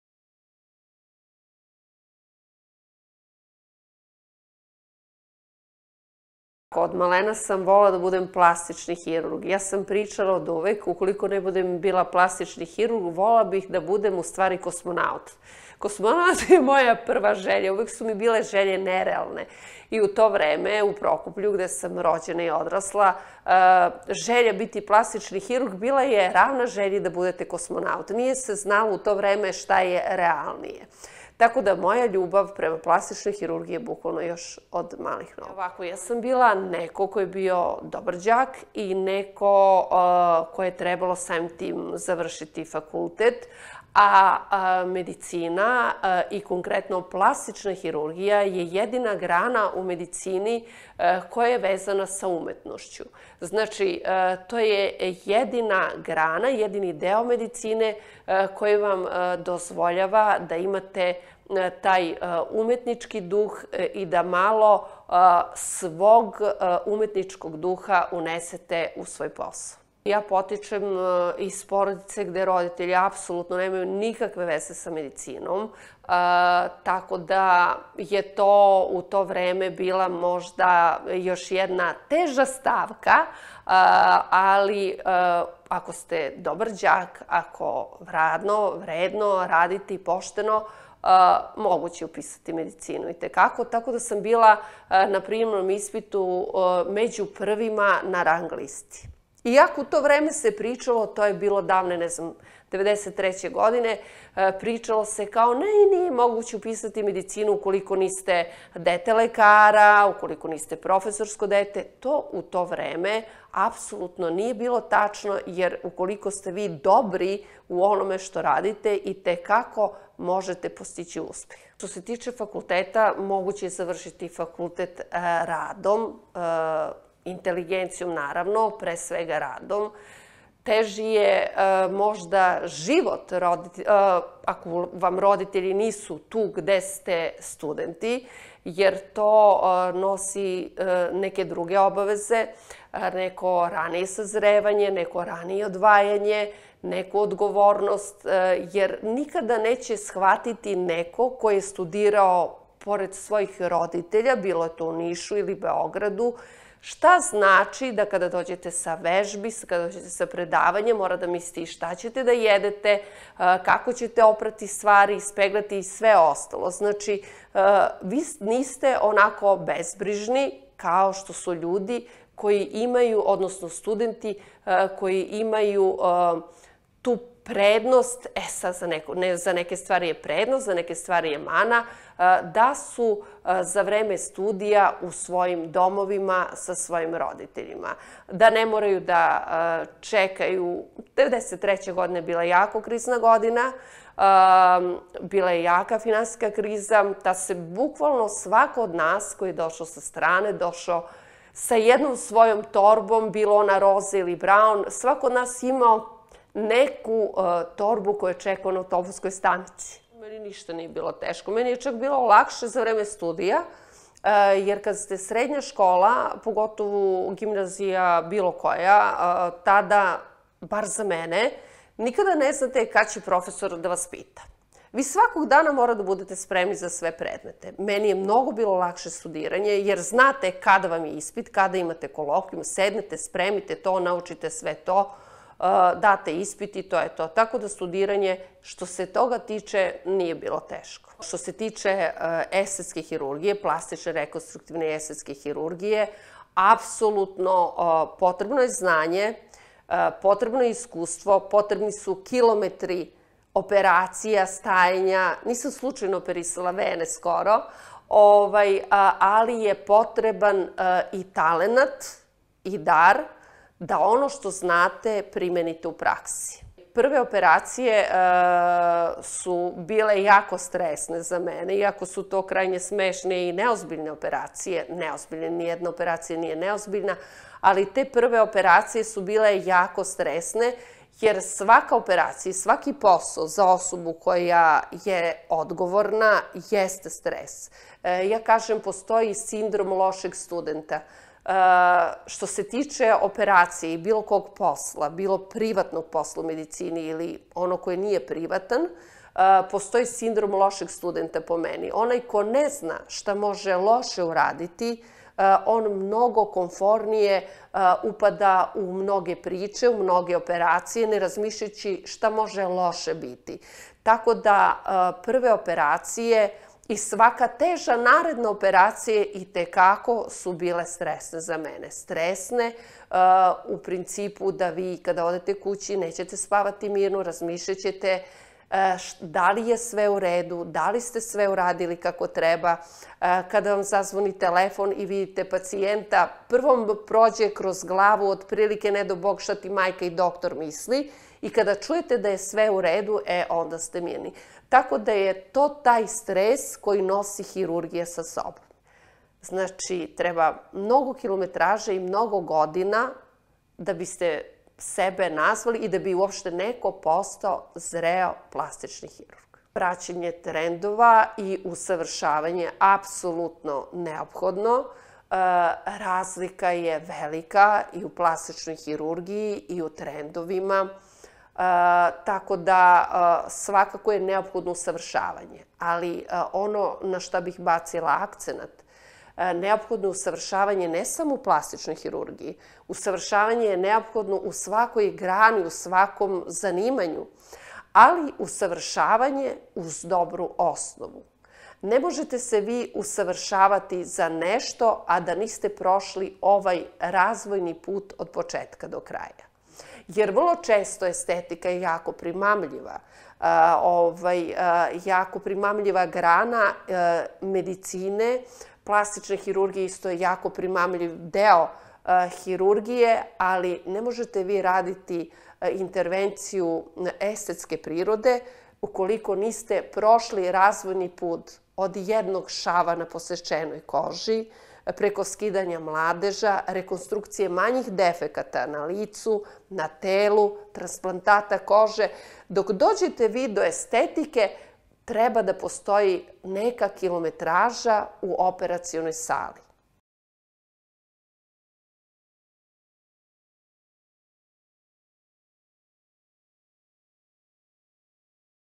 Dr Janković: Plastična hirurgija je umetnost
Intervju: Dr Irena Janković, specijalista plasticne, rekonstruktivne i estetske hirurgije